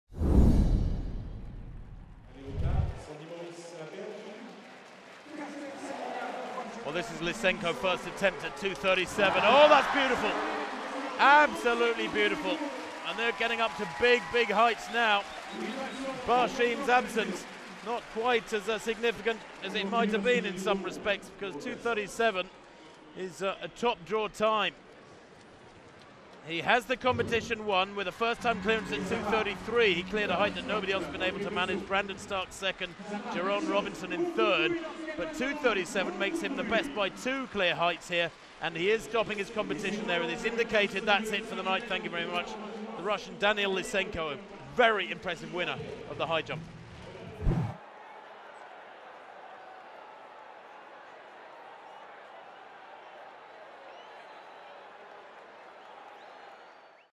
Danil Lysenko 2.37 Wins Men's High Jump - IAAF Diamond League Lausanne 2018
Top 3 results:

1. Danil Lysenko (Neutral) - 2.37 SB
2. Brandon Starc (Australia) - 2.29
3. Jeron Robinson (USA) - 2.29